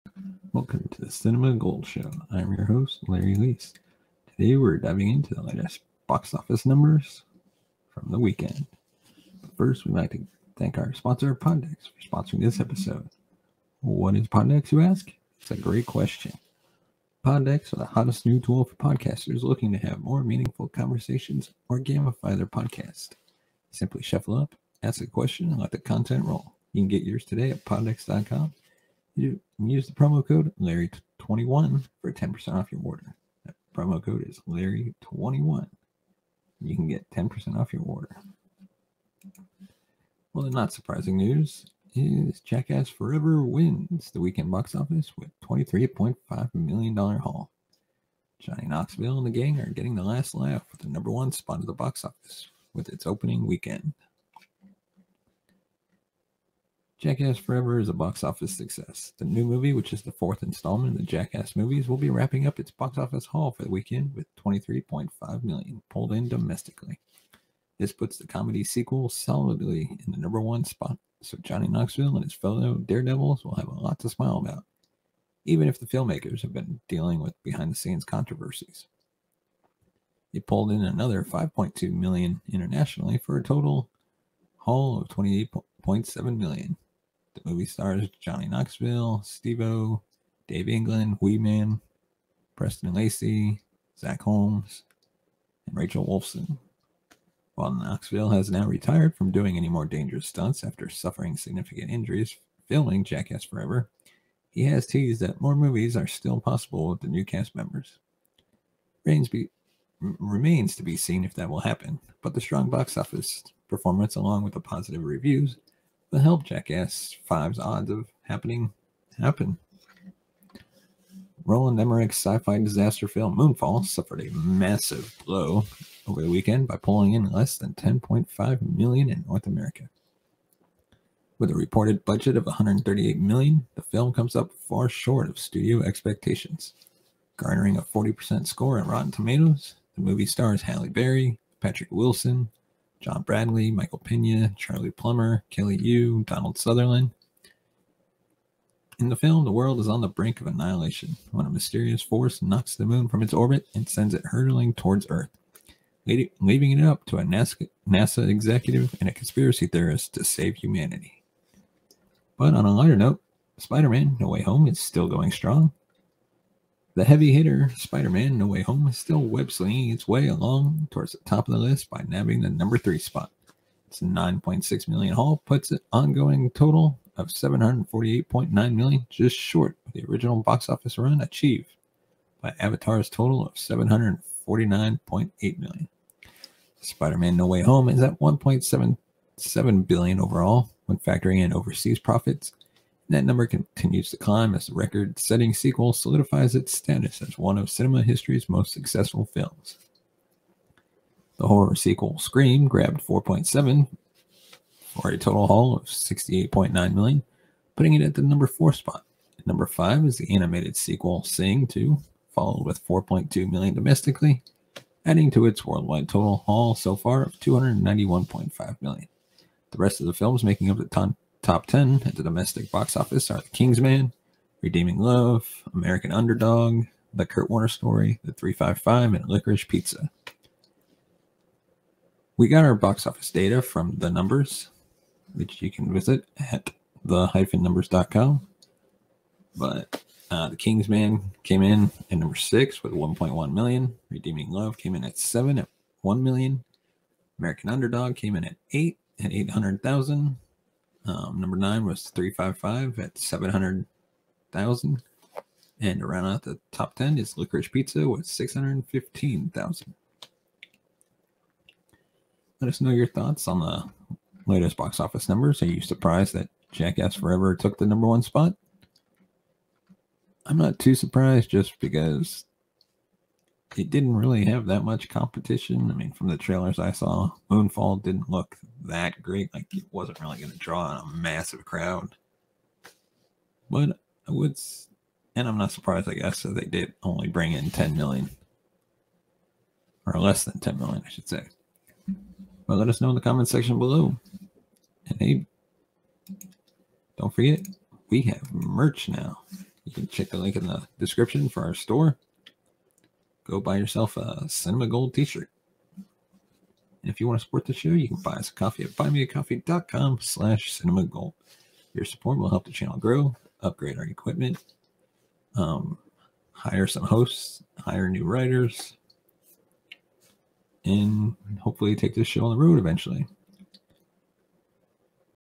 Box Office Report: Jackass Forever Wins Weekend Box Office With $23.5 Million Haul
Jackass Forever is a box office success. The new movie, which is the fourth installment of the Jackass movie series, will be wrapping up its box office haul for the weekend with $23.5 million pulled in domestically. This puts the comedy sequel solidly in the No. 1 spot so Johnny Knoxville and his fellow daredevils will have a lot to smile about, even if the filmmakers have been dealing with behind-the-scenes controversies. It pulled in another $5.2 million internationally for a total haul of $28.7 million.